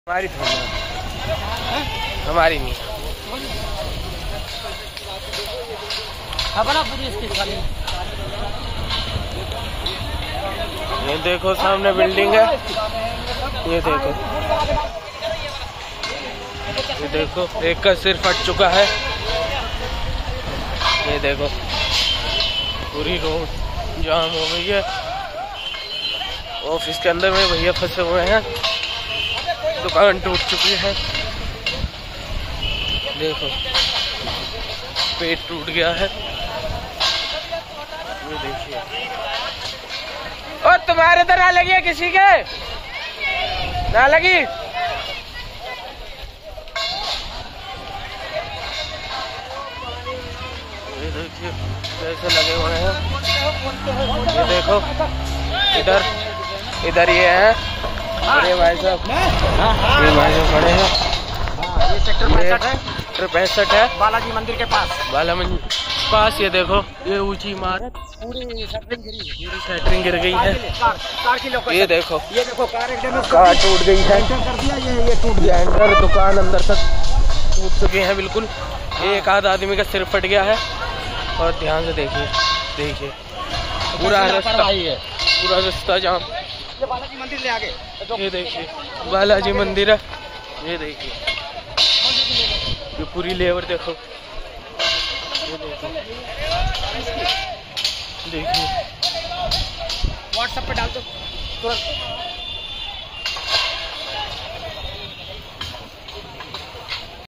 निर्माणाधीन बिल्डिंग की सेट्रिंग गिरी जो सड़क व मार्केट पर आकर गिरी जिससे कई लोग दुकान के फसे रहे
सेक्टर 65 में निर्माणाधीन बिल्डिंग की सेट्रिंग गिरी जो सड़क व मार्केट पर आकर गिरी जिससे कई लोग दुकान के फसे रहे